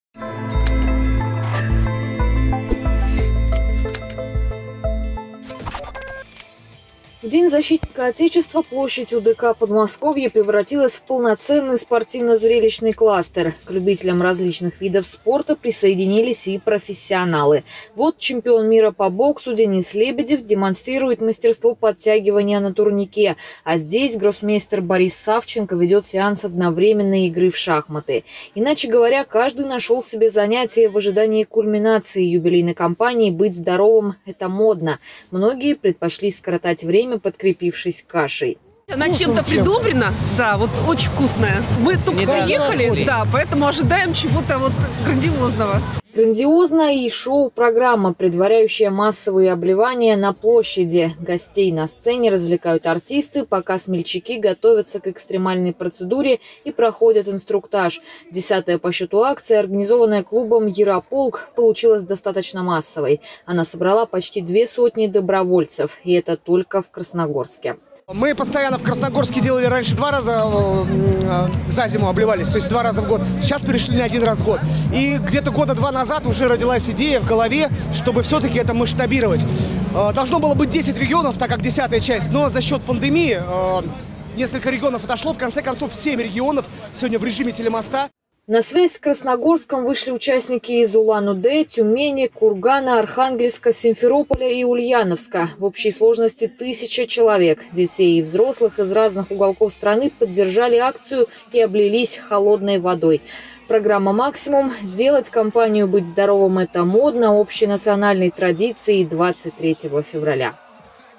Репортаж телеканала КРТВ, Всероссийская акция ЗОЖ от клуба ЯРОПОЛК, город Красногорск, 23.02.2022 г.
Около тысячи человек поучаствовали в первом Всероссийском массовом закаливании, которое организовал спортивно-патриотический клуб "ЯРОПОЛК".
В День защитника Отечества Подмосковный Красногорск стал центром уникальной Всероссийской спортивно-патриотической акции «Быть здоровым – это модно 10!».
К этому семейному празднику спорта и здорового образа жизни присоединилось около тысячи человек из семи регионов России. Мероприятие прошло в формате телемоста между участниками акции из Красногорска, Тюмени, Симферополя, Ульяновска, Кургана, Архангельска и Улан-Уде.
Главным событием мероприятия стала уникальна акция — все участники телемоста одновременно опрокинули на себя вёдра с ледяной водой.

Мероприятие организованно спортивно-патриотическим клубом «ЯРОПОЛК» при поддержке Министерства спорта РФ, а также администрации городского округа Красногорск.